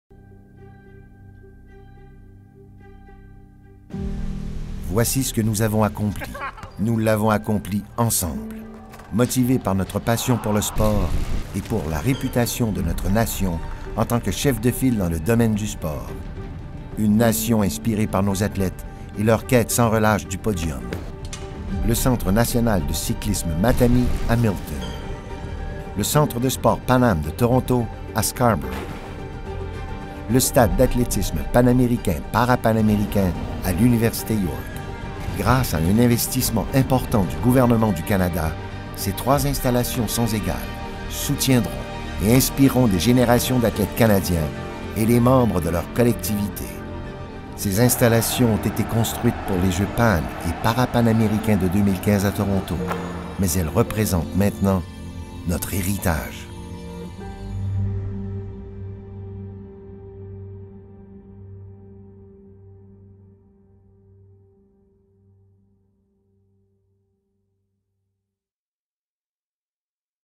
Legs des jeux - Jeux panaméricains et parapanaméricains de 2015 à Toronto
Le gouvernement du Canada investit jusqu’à 442,1 millions de dollars dans 27 installations sportives, y compris 10 nouvelles installations, dans les cadre des Jeux panaméricains et parapanaméricains de 2015 à Toronto. Il s’agit d’un legs durable pour nos athlètes et pour les communautés où se trouvent ces installations.